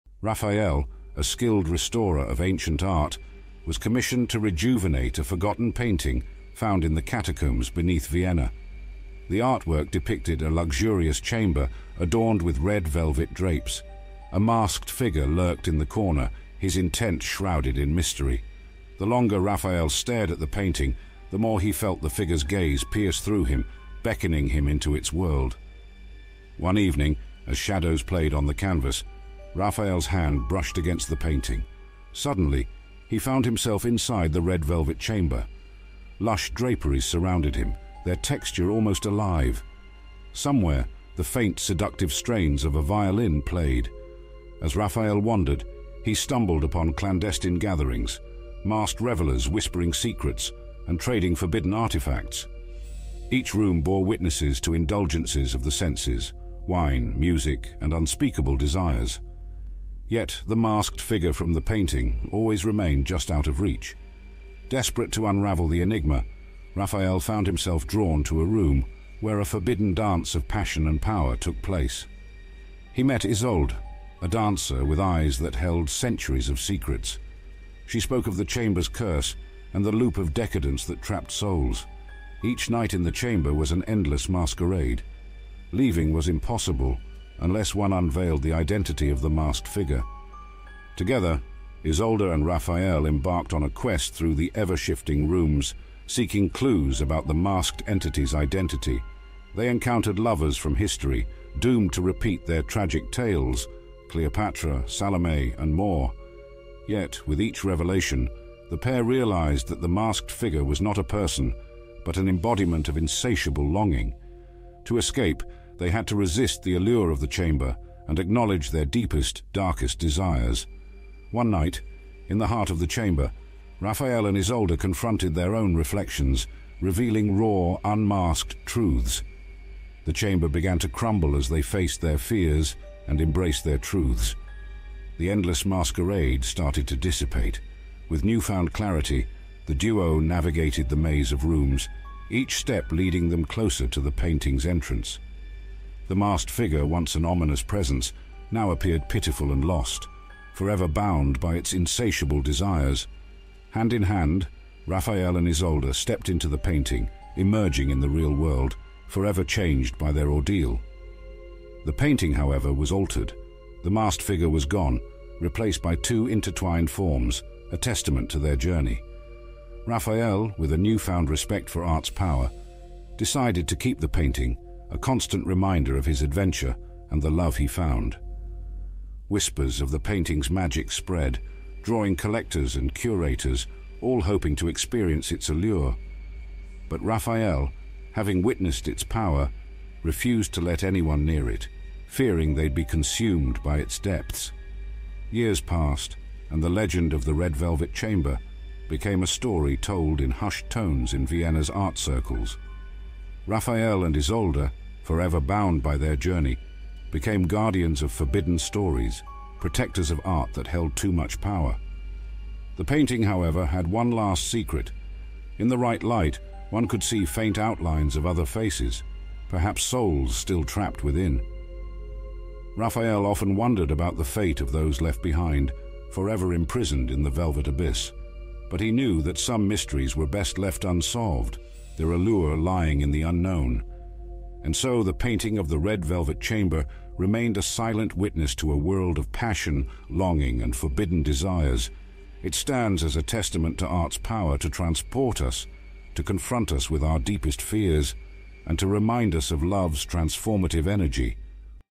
Stories For Grown Ups - Whispers in the Red Velvet Chamber
Stories For Grown Ups - 🎭 Journey with Raphael into the enigmatic realms of art, where each brushstroke hides a world of passion, mystery, and desire. Can love be the key to escape an eternal masquerade? Dive into Vienna's art circles and uncover the legend of the Red Velvet Chamber. 🎨